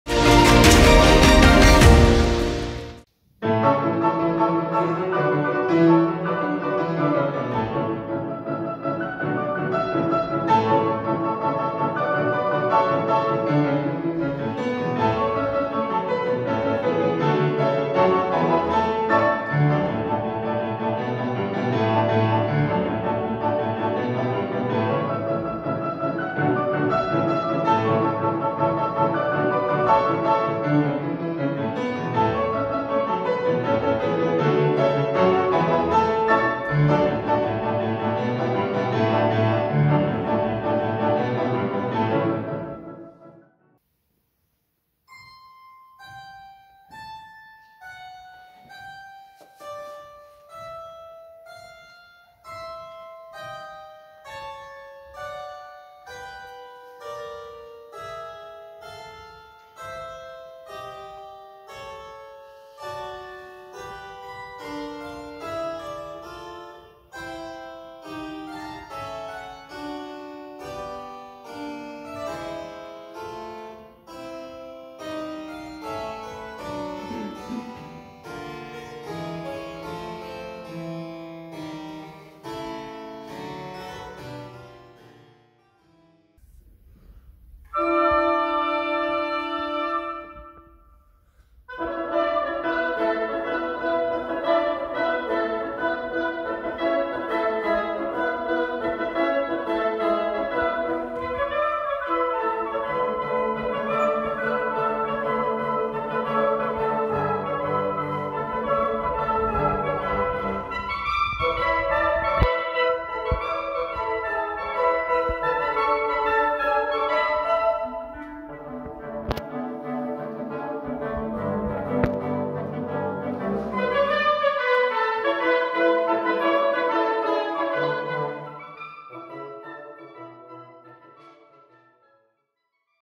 A 100 éve született Ligeti Györgyre emlékeztek a koncerttel Kecskeméten
Valódi zenei csemegével készült a zenebarátoknak a Kodály Iskola hétfő este, amikor a 100 éve született Ligeti György Kossuth-díjas, világhírű zeneszerző műveiből adtak kamarakoncertet a művészek. Részletek